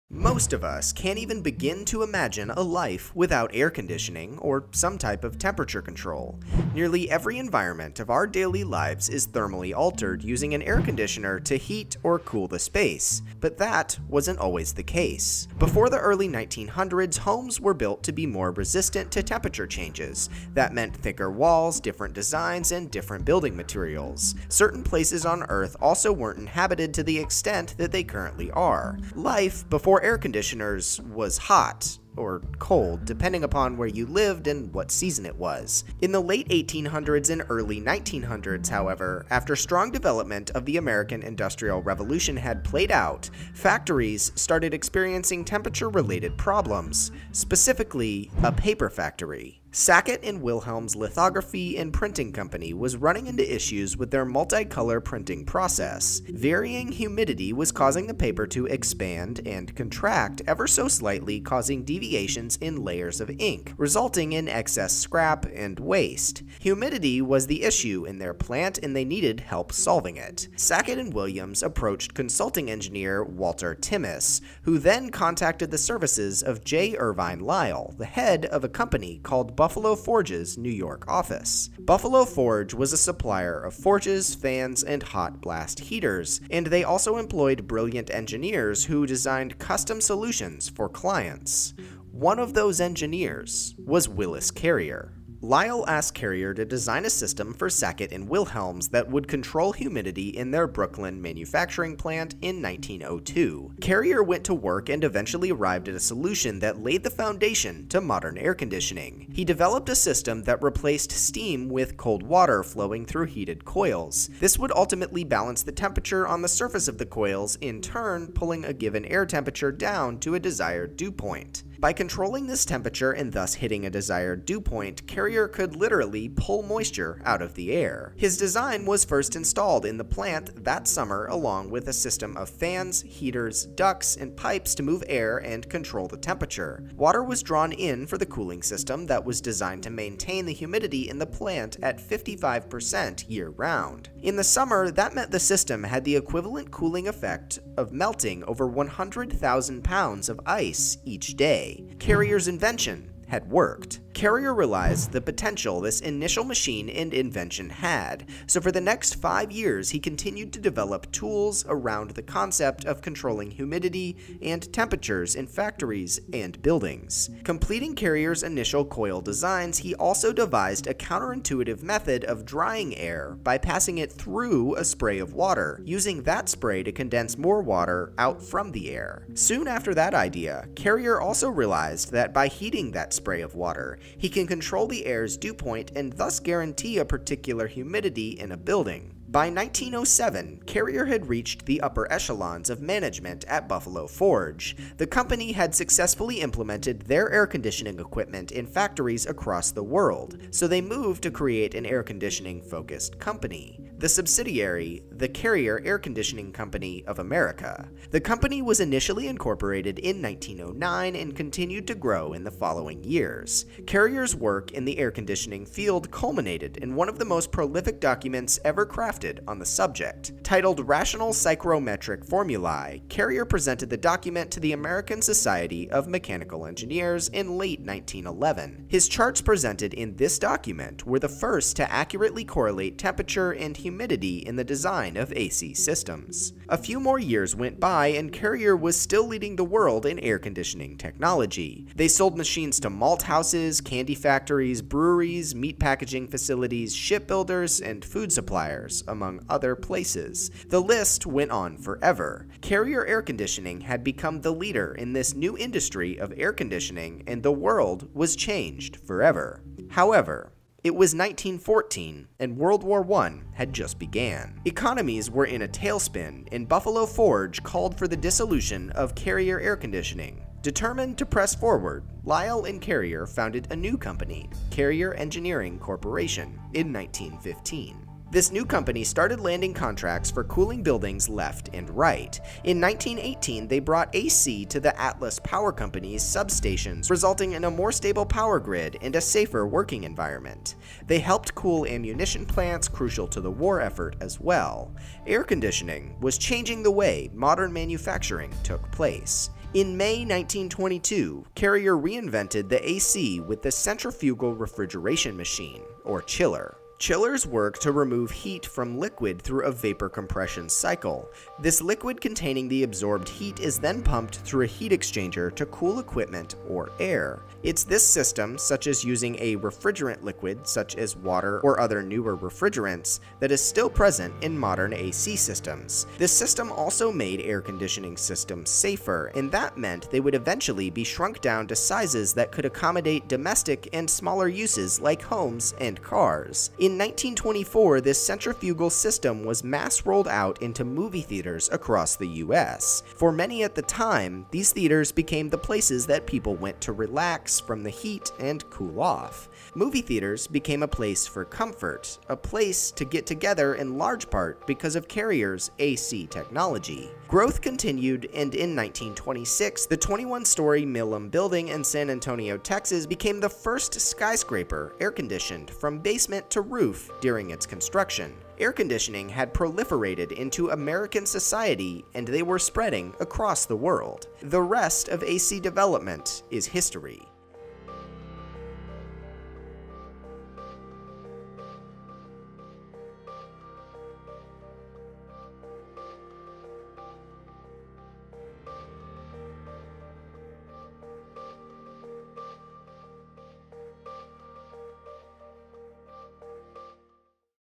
How does Air Conditioning Work?
Most of us can't even begin to imagine a life without air conditioning or some type of temperature control. Nearly every environment of our daily lives is thermally altered using an air conditioner to heat or cool the space – but this wasn't always the case.

Before the early 1900s, homes were built to be more resistant to temperature changes. That meant thicker walls, different designs, and different building materials. Certain places on earth also weren't inhabited to the extent that they currently are. Life before air conditioners was hot... or cold... depending upon where you lived and what season it was.

In the late 1800s and early 1900s, however, after the strong development of the American Industrial Revolution had played out, factories started experiencing temperature related problems. Specifically, paper factories.